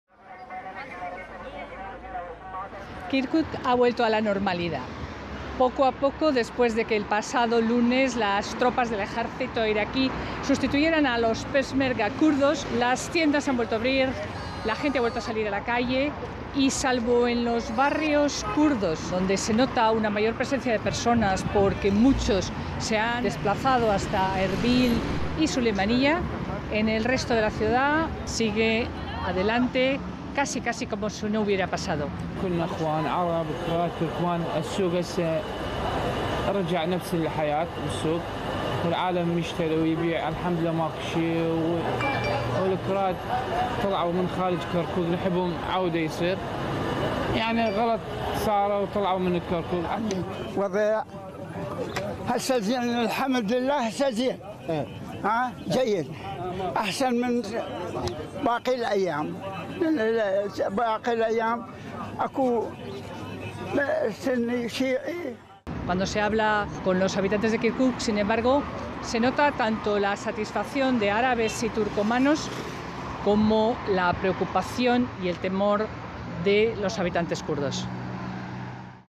Kirkuk vuelve a la normalidad | Internacional
Kirkuk vuelve a la normalidad después de la actuación del pasado lunes de las tropas del ejército irakí